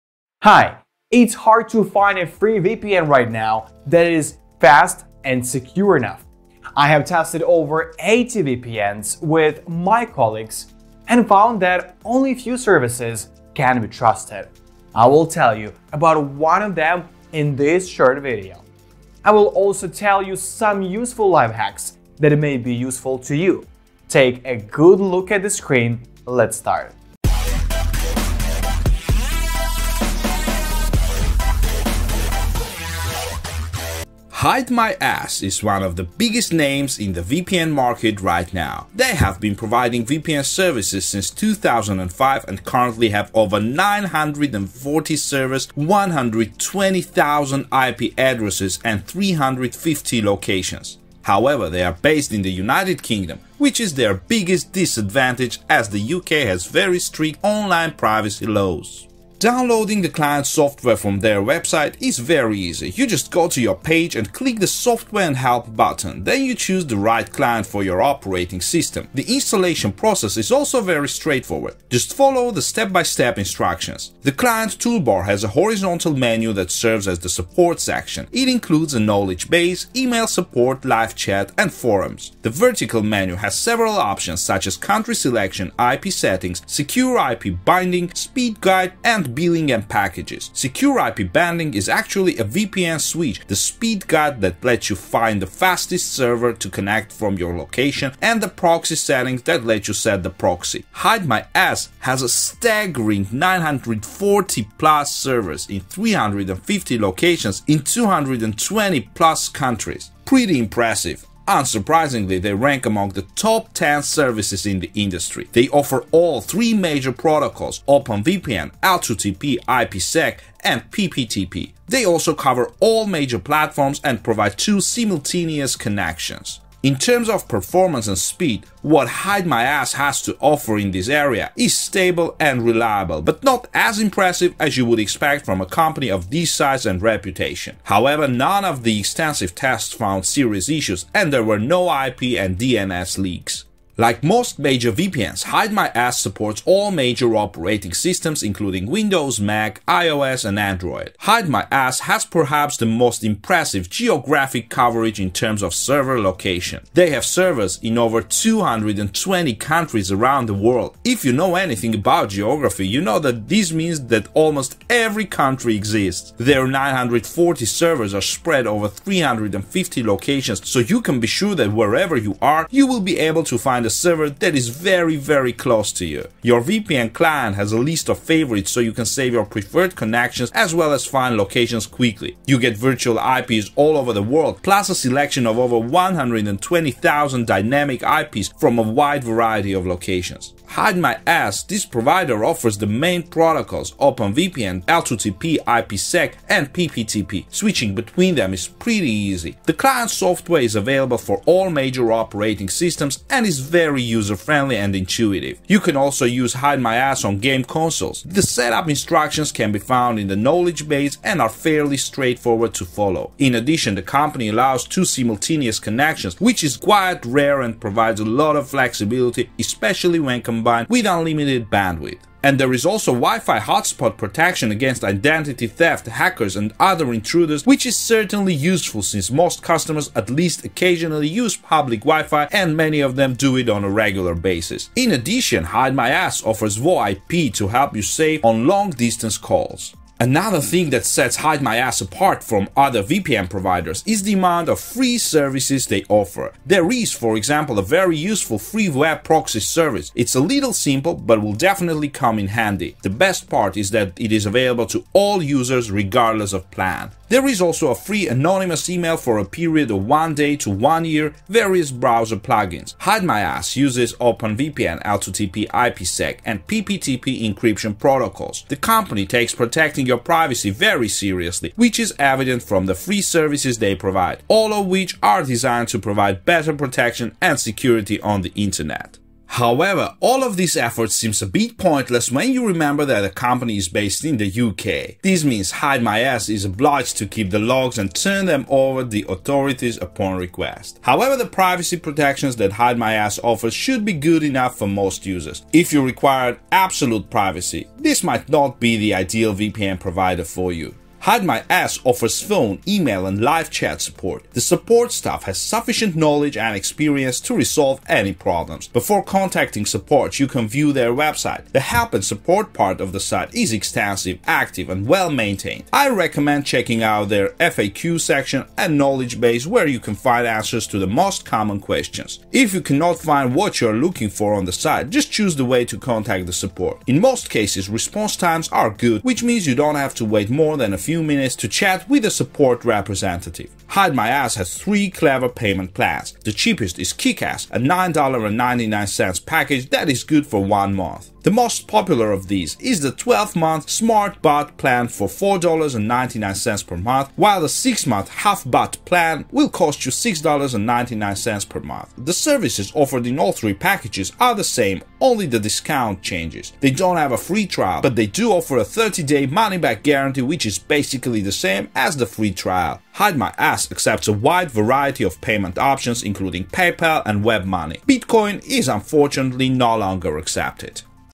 Best Free VPN Service For PC Review 2021🔥
Hello, I want to share with you my experience of the most reliable Top VPN service.  After testing many privacy systems, I found the best one that worked perfectly for me.  I've been using this VPN for over three years now.  In this video, I'll reveal to you five very useful secrets that few people know about, but which make it truly the best Top VPN on the market.  It's about HideMyAss.  Look carefully at the screen, I'll show you everything with a clear example!